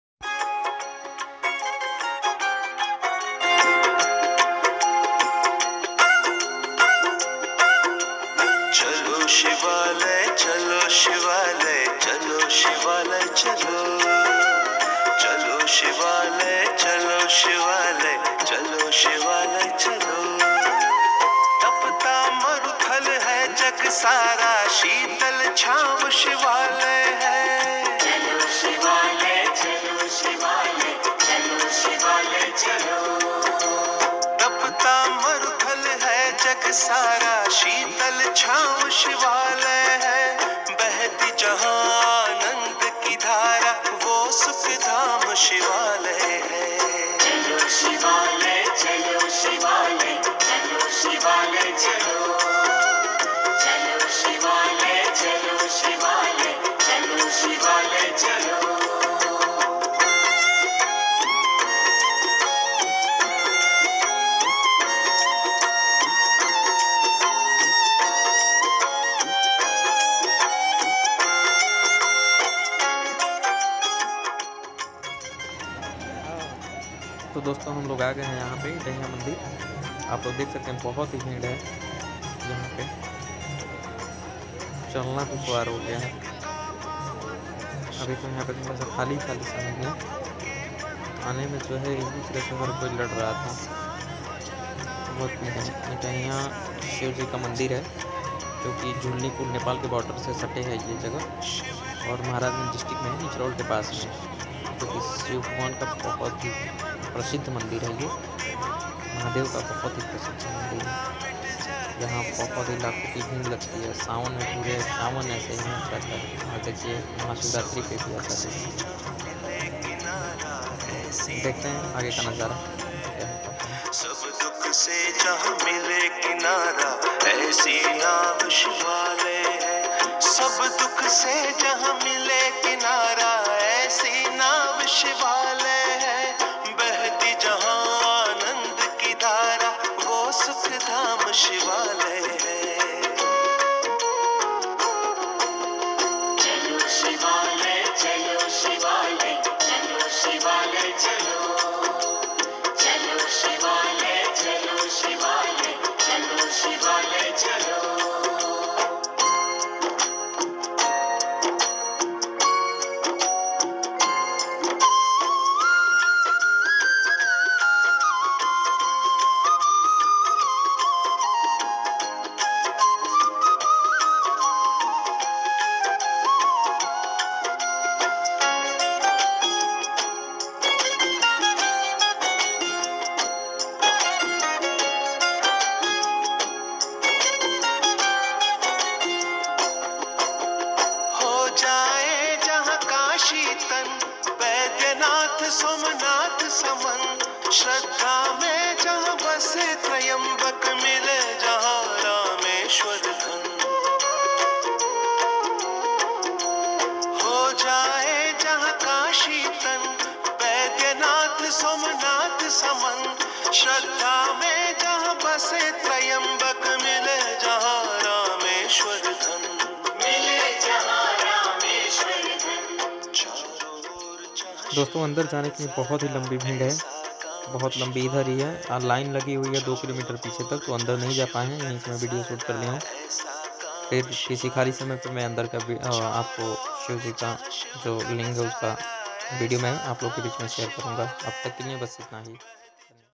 महाशिवरात्रि 2022 करें महादेव का दर्शन//new vlog
हैल्लो दोस्तों 👋
आज महाशिवरात्रि के पावन दिन पे आप सभी दर्शकों को मेरा प्यार भरा नमस्कार 😊
दोस्तों ये वीडियो गोरखपुर मण्डल के महराजगंज जिला में स्थित इटहिया मंदिर का है जो महादेव जी का बहुत ही प्रशिद्ध मंदिर है यहाँ महादेव जी का लिंग जमीन के निचे से प्रकट है जो हमेसा ही बड़े होते रहते है! यहाँ श्रद्धांलूवो का लाखो की संख्या में भीड़ उमड़ता है अगर आप लोगो को यहाँ के बारे में ज्यादा जानकारी चाहिए तो हमें कमेंट कर सकते है! और ये वीडियो लाइक जरूर करें 👍 महादेव की कृपा सदैव सबपे बनी रहे हर हर महादेव 🙏